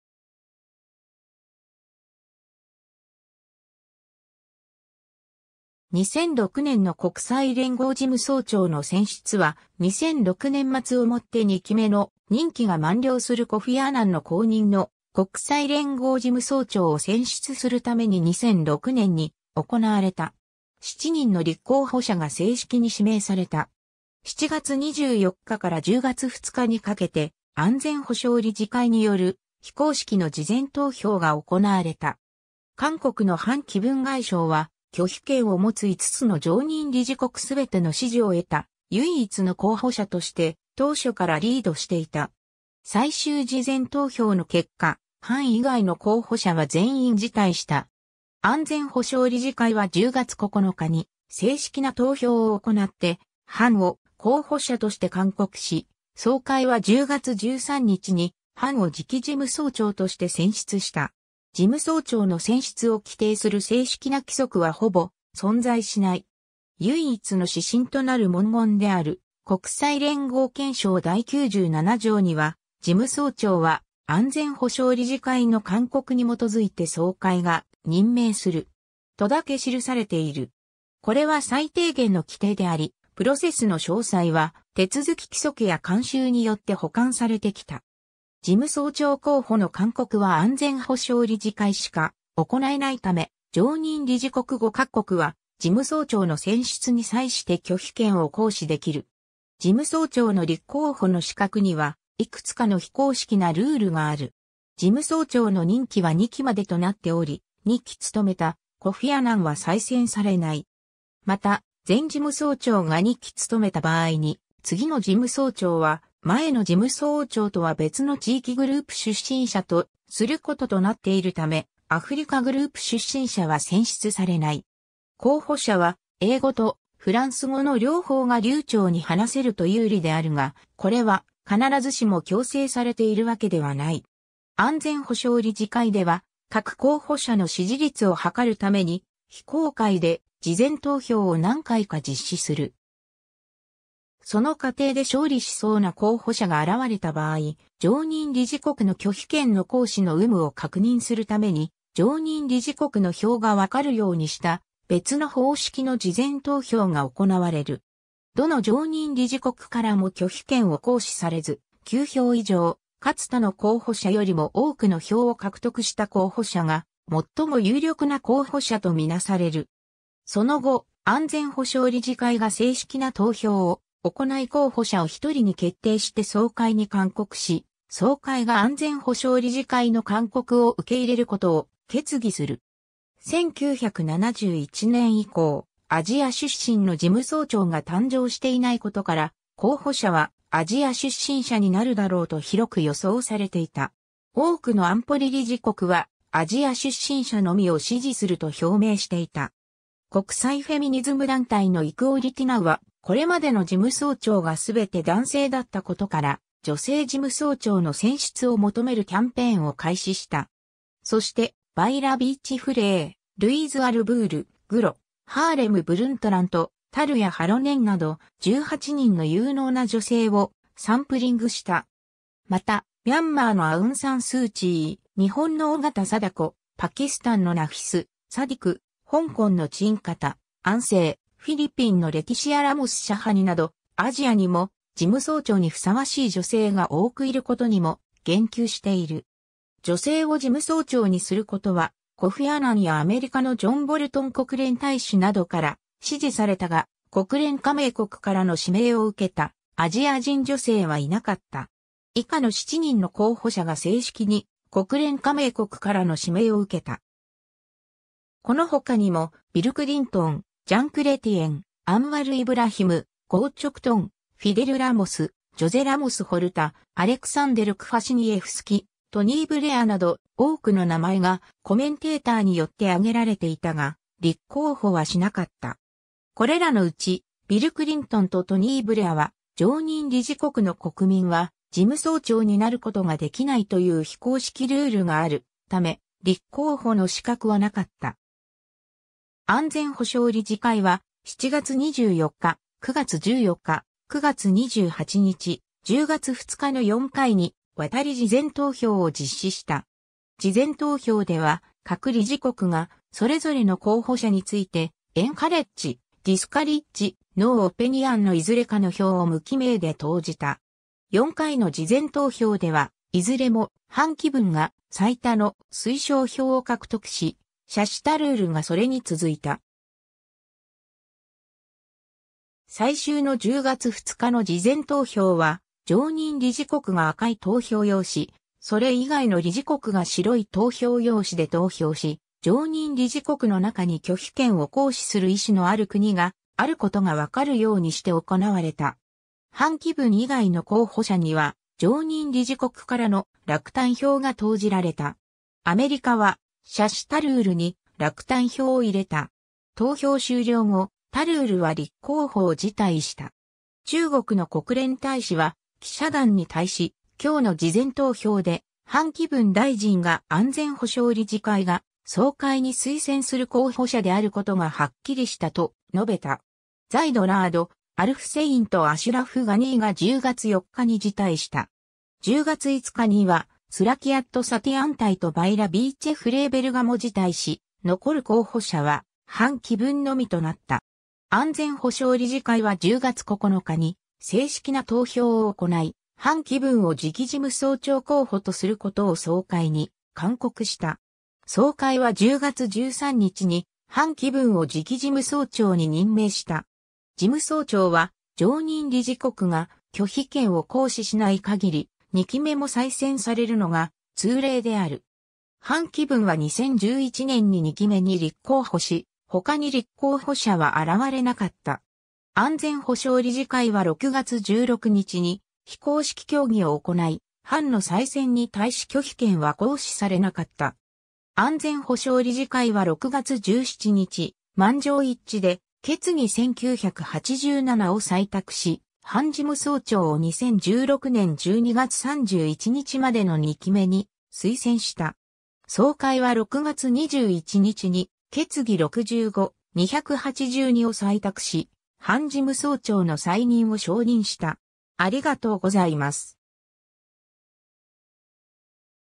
2006年の国際連合事務総長の選出
2006年の国際連合事務総長の選出は、2006年末を以て2期目の任期が満了するコフィ・アナンの後任の国際連合事務総長を選出するために2006年に行われた。
7人の立候補者が正式に指名された。
 7月24日から10月2日にかけて、安全保障理事会による非公式の事前投票が行われた。
韓国の潘基文外相は、拒否権を持つ5つの常任理事国(P5)全ての支持を得た唯一の候補者として、当初からリードしていた。
最終事前投票の結果、潘以外の候補者は全員辞退した。
安全保障理事会は10月9日に正式な投票を行って潘を候補者として勧告し、総会は10月13日に潘を次期事務総長として選出した。
 事務総長の選出を規定する正式な規則はほぼ存在しない。
唯一の指針となる文言である国際連合憲章第97条には、「事務総長は、安全保障理事会の勧告に基いて総会が任命する。」とだけ記されている。
これは最低限の規定であり、プロセスの詳細は手続き規則や慣習によって補完されてきた。
事務総長候補の勧告は安全保障理事会しか行えないため、常任理事国5か国(P5)は事務総長の選出に際して拒否権を行使できる。
 事務総長の立候補の資格には、いくつかの非公式なルールがある。
事務総長の任期は2期までとなっており、2期務めたコフィ・アナンは再選されない。
また、前事務総長が2期（以上）務めた場合に、次の事務総長は前の事務総長とは別の地域グループ出身者とすることとなっているため、アフリカグループ出身者は選出されない。
候補者は英語とフランス語の両方が流暢に話せると有利であるが、これは必ずしも強制されているわけではない。
 安全保障理事会では、各候補者の支持率を測るために、非公開で事前投票を何回か実施する。
その過程で勝利しそうな候補者が現れた場合、常任理事国の拒否権の行使の有無を確認するために、常任理事国の票がわかるようにした別の方式の事前投票が行われる。
どの常任理事国からも拒否権を行使されず、9票以上かつ他の候補者よりも多くの票を獲得した候補者が、最も有力な候補者とみなされる。
その後、安全保障理事会が正式な投票を行い候補者を1人に決定して総会に勧告し、総会が安全保障理事会の勧告を受け入れることを決議する。
 1971年以降、アジア出身の事務総長が誕生していないことから、候補者はアジア出身者になるだろうと広く予想されていた。
多くの安保理理事国（拒否権を持つ中国を含む）は、アジア出身者のみを支持すると表明していた。
 国際フェミニズム団体のイクオリティ・ナウは、これまでの事務総長が全て男性だったことから、女性事務総長の選出を求めるキャンペーンを開始した。
そして、ヴァイラ・ヴィーチェ＝フレイベルガ、ルイーズ・アルブール、グロ・ハーレム・ブルントラント、タルヤ・ハロネンなど、18人の有能な女性を「サンプリング」した。
また、ミャンマーのアウンサンスーチー、日本の緒方貞子、パキスタンのナフィス・サディク、香港の陳方安生（アンソン・チャン）、フィリピンのレティシア・ラモス＝シャハニなど、アジアにも事務総長にふさわしい女性が多くいることにも言及している。
女性を事務総長にすることは、コフィ・アナンやアメリカのジョン・ボルトン国連大使などから支持されたが、国連加盟国からの指名を受けたアジア人女性はいなかった。
 以下の7人の候補者が正式に国連加盟国からの指名を受けた。
 この他にも、ビル・クリントン（元アメリカ大統領）、ジャン・クレティエン（元カナダ首相）、アンワル・イブラヒム（元マレーシア副首相）、ゴー・チョクトン（シンガポール上級大臣、元首相）、フィデル・ラモス（元フィリピン大統領）、ジョゼ・ラモス＝ホルタ（東ティモール首相）、アレクサンデル・クファシニェフスキ（元ポーランド大統領）、トニー・ブレア（イギリス首相）など、多くの名前がコメンテーターによって挙げられていたが、立候補はしなかった。
これらのうち、ビル・クリントンとトニー・ブレアは、常任理事国の国民は事務総長になることができないという非公式ルールがあるため、立候補の資格はなかった。
 安全保障理事会は、7月24日、9月14日、9月28日、10月2日の4回にわたり事前投票を実施した。
事前投票では、各理事国がそれぞれの候補者について、"encourage"（推奨する）、"discourage"（落胆する）、"no opnion"（...